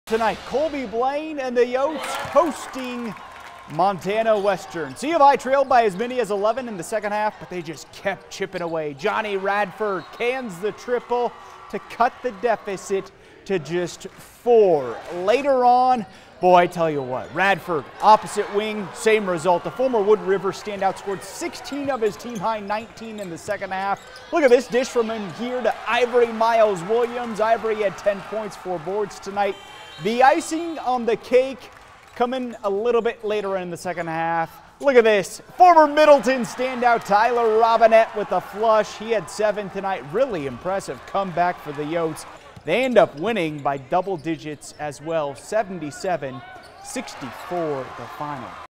Highlights: College of Idaho vs. Montana Western basketball 11/19/2021
College of Idaho vs. Montana Tech men's basketball highlights from Caldwell.